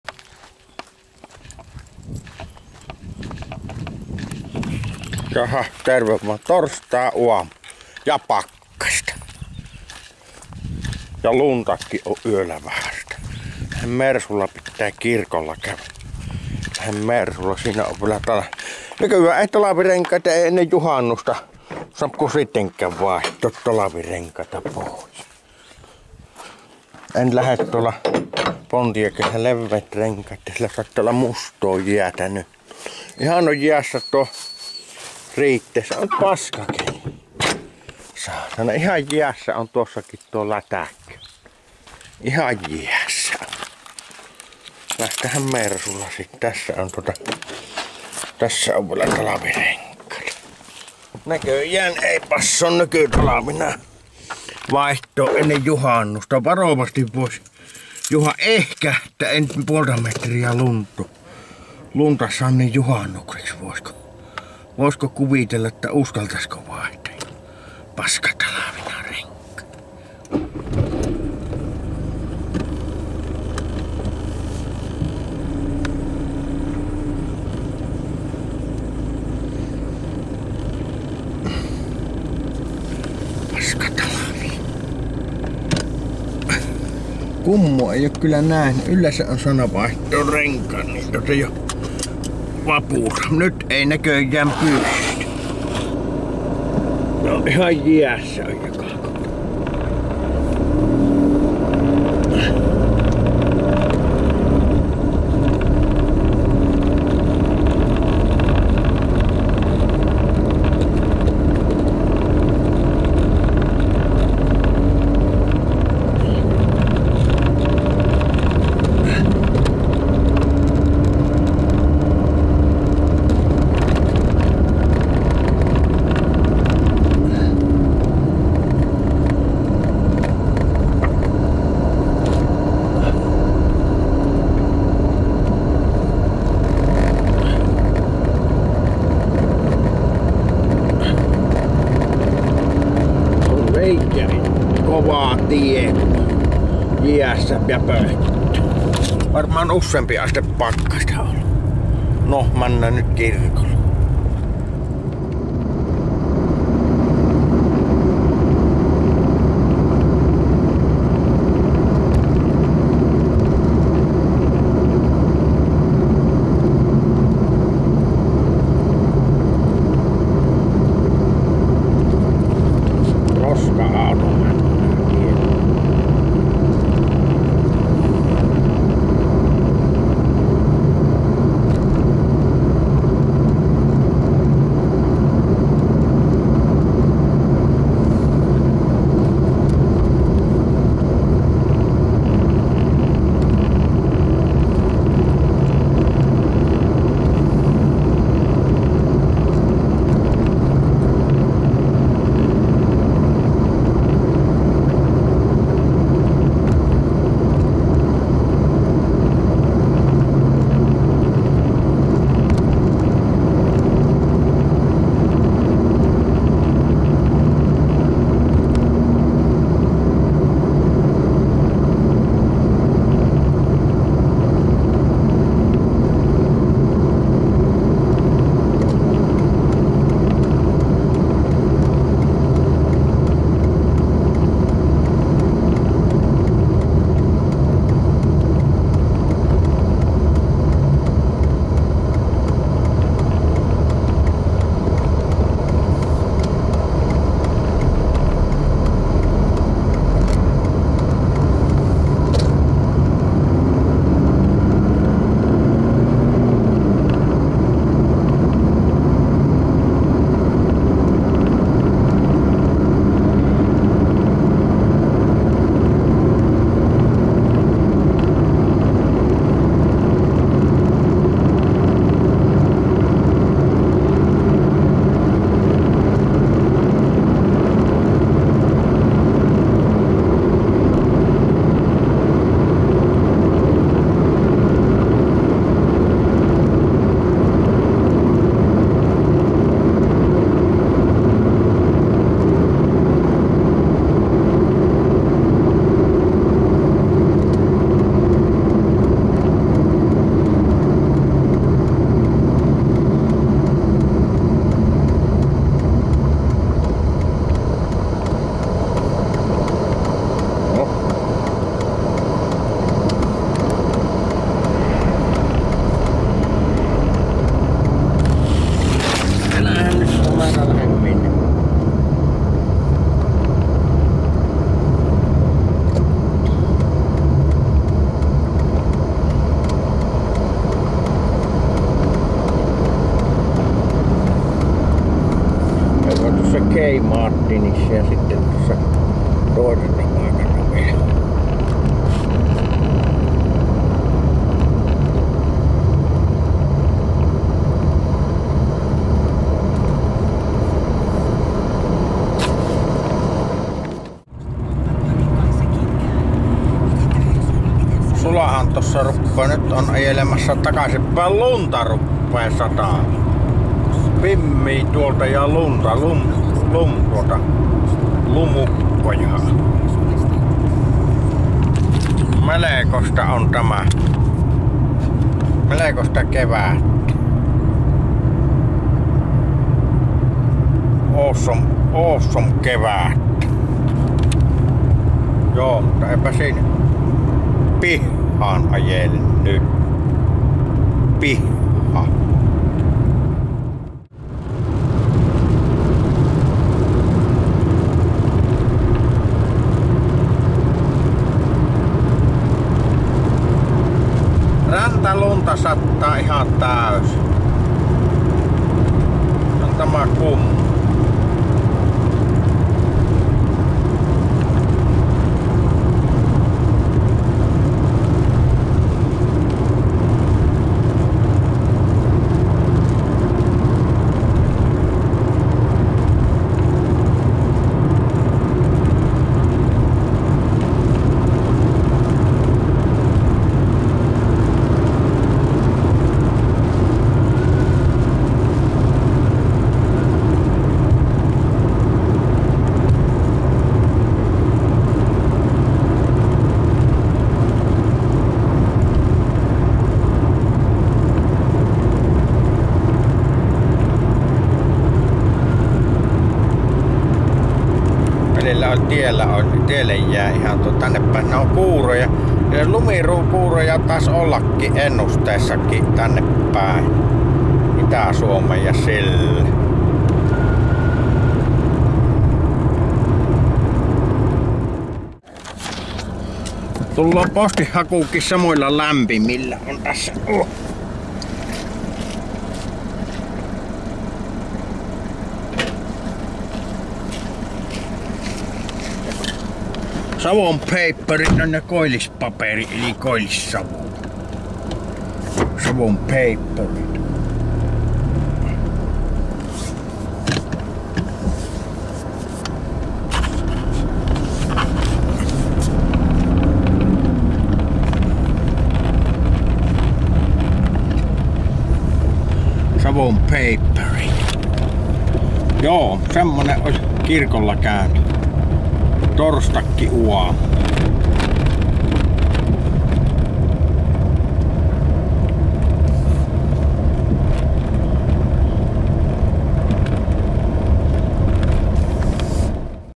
Kirkolle TORSTAINA Mersulla
Kirkonkylälle asioille.. luntakii sataa.. !!